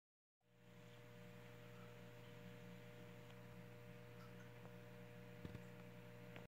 My 225 Gallon Reef Tank: 4/28/16
Very short clip off the new tank.. Water is cloudy because of some calcium precipitate but definitely clearing up.  Tank isn't fully plumbed yet hence why the water is still below the overflows.